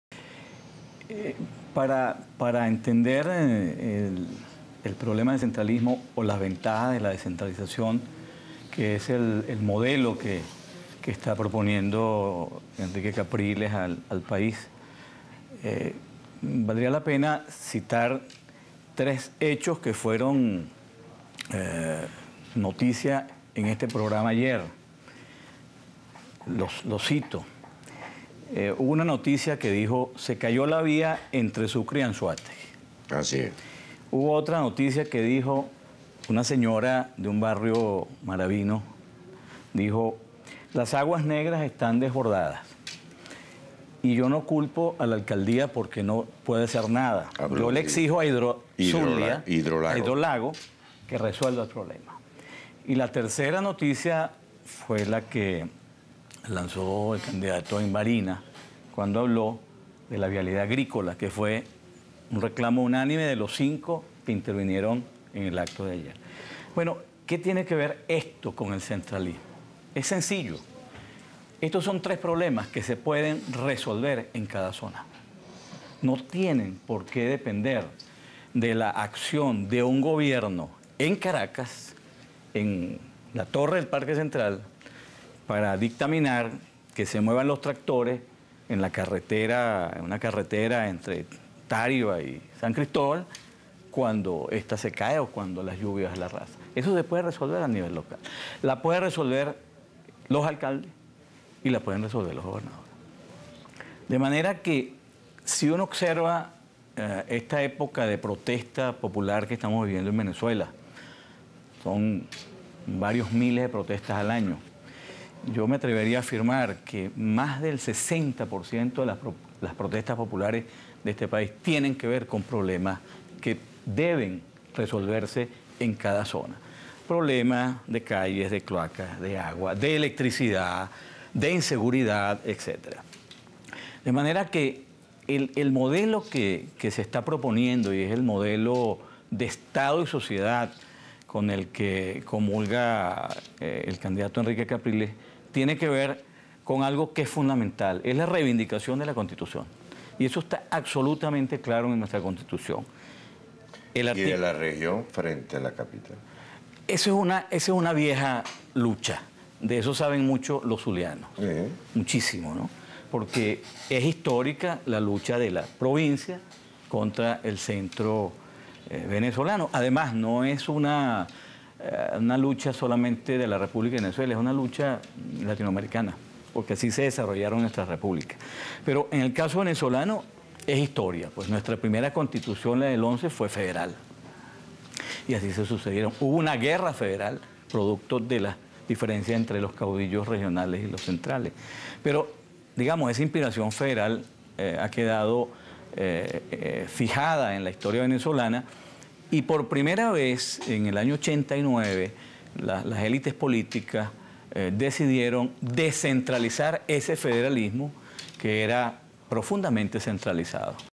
Denuncian que Gobierno violó sentido federalista y descentralizado de la Constitución
El profesor Carlos Mascareño, vocero del Comando Venezuela de Henrique Capriles Radonski, indicó que los problemas de vialidad, seguridad y servicios básicos se pueden resolver por la acción de alcaldes y gobernadores si existiese una descentralización real